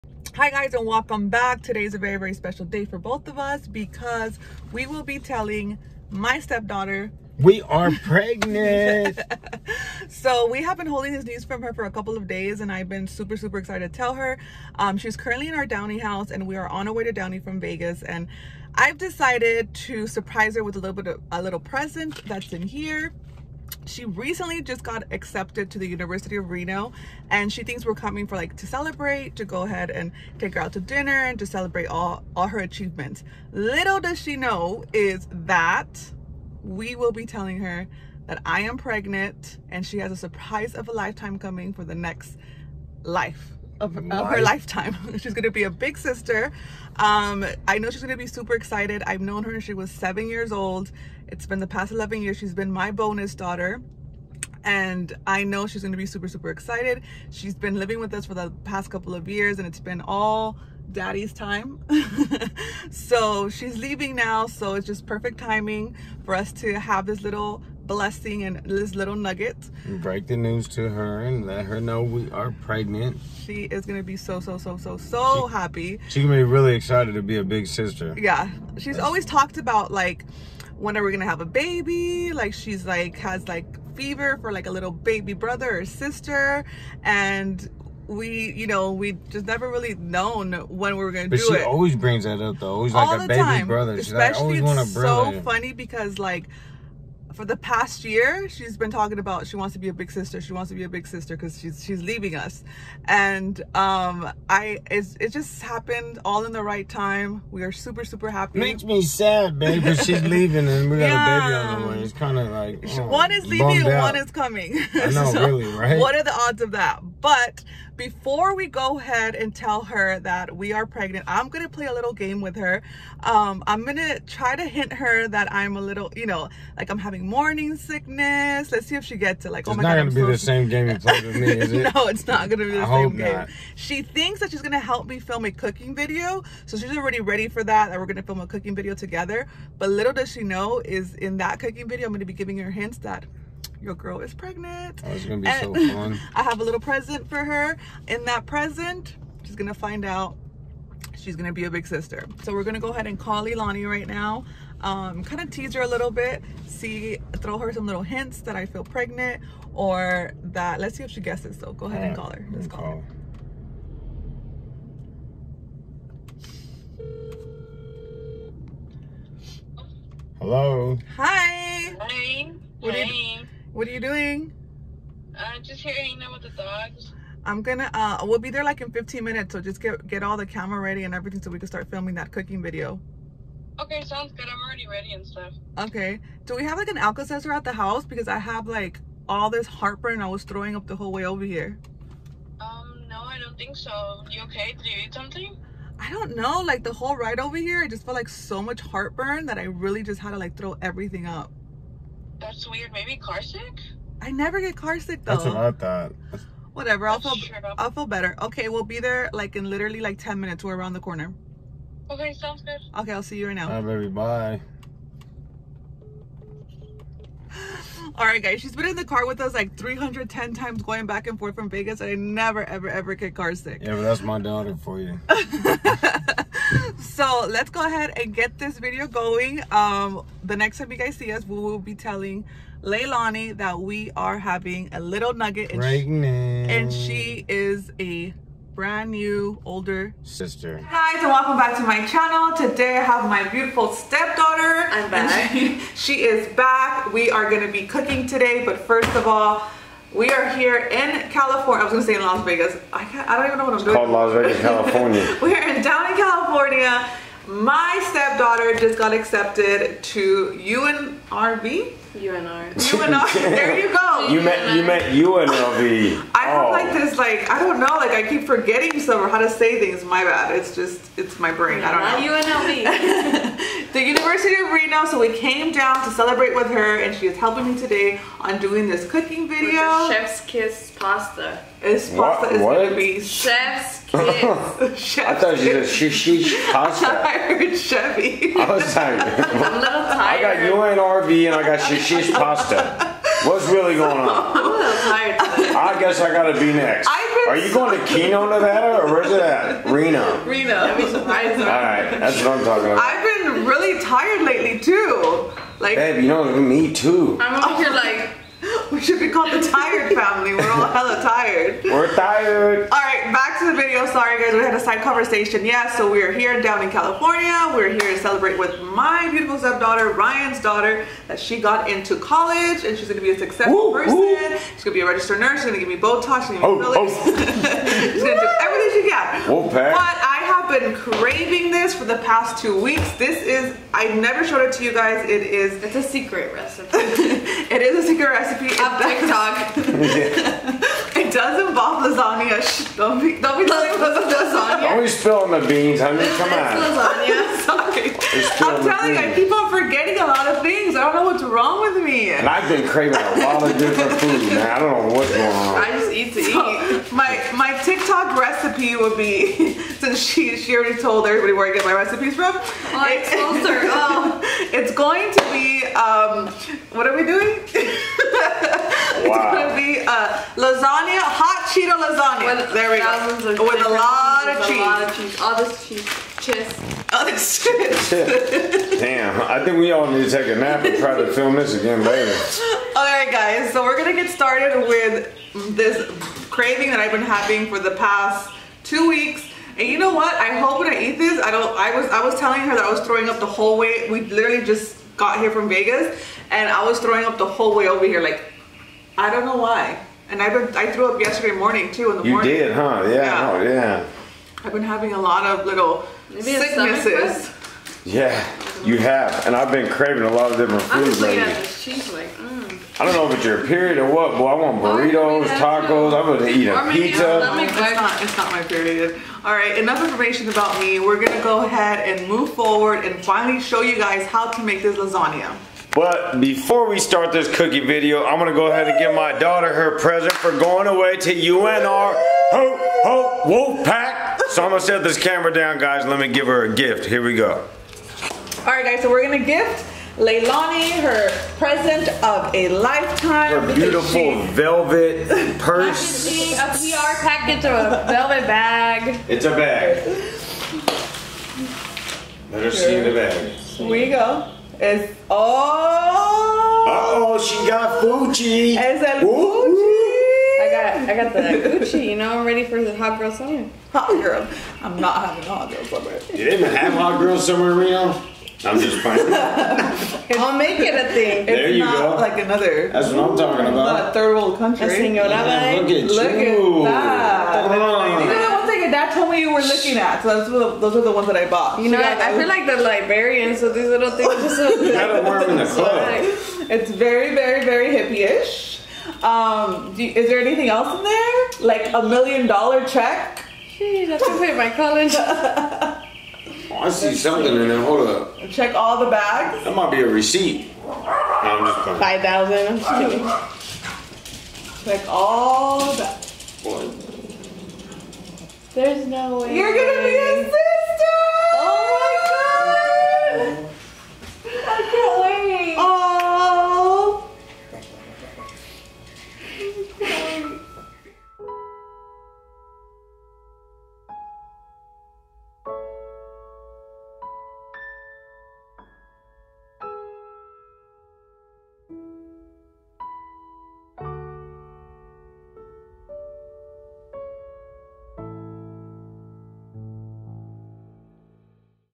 TELLING MY FAMILY I'M PREGNANT!
Telling my stepdaughter I am pregnant was a very special moment for me! I am so happy I am able to share  my baby journey with all of you! Thank you for all the love and support! LOVE YOU GUYS!

Dont forget to turn on you post notification so you can get notify every time I upload!